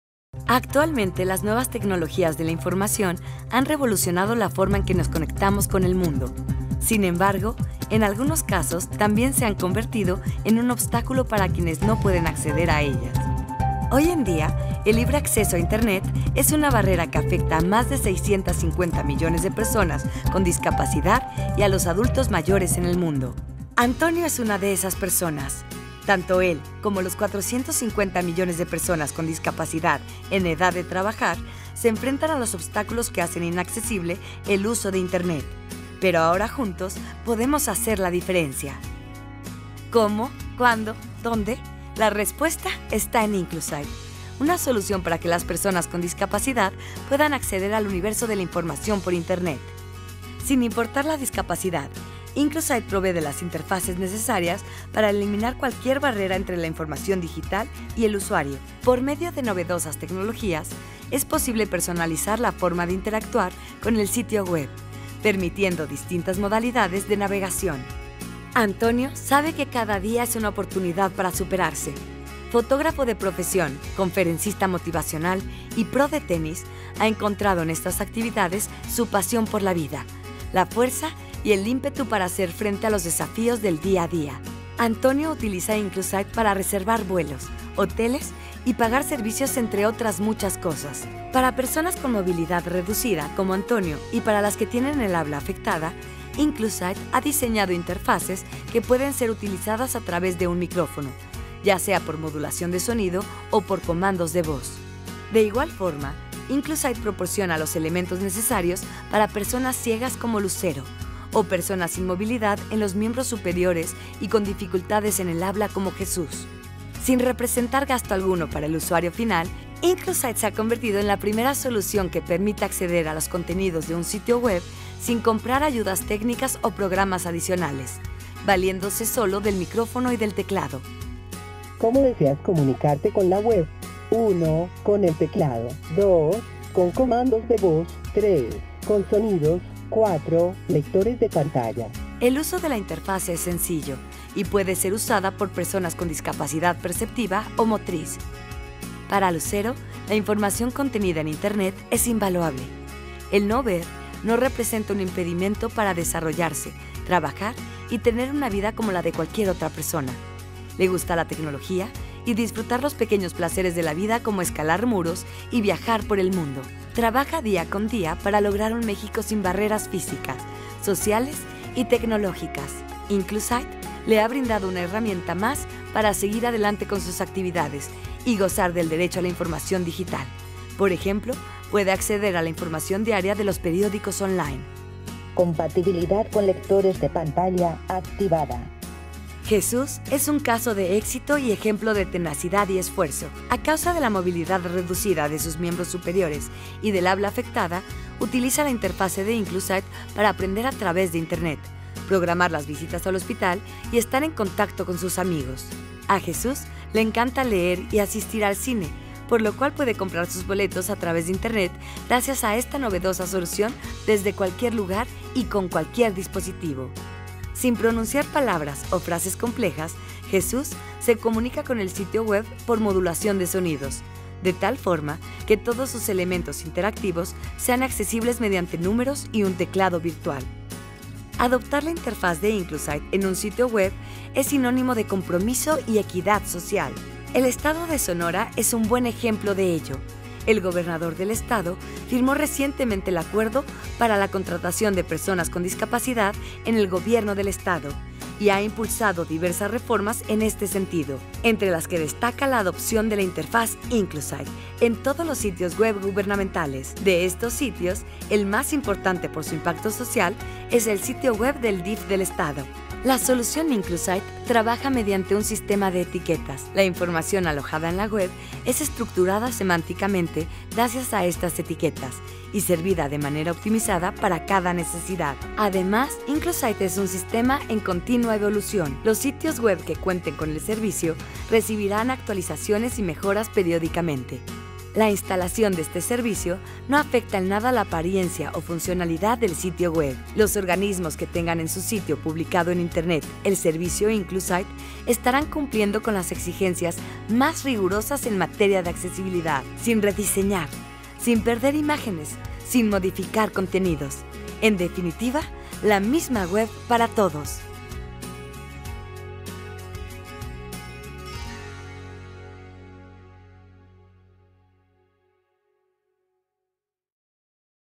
INCLUSITE le pone rampas a la web
Inclusite es una solución para que las personas con discapacidad, puedan tener acceso a internet.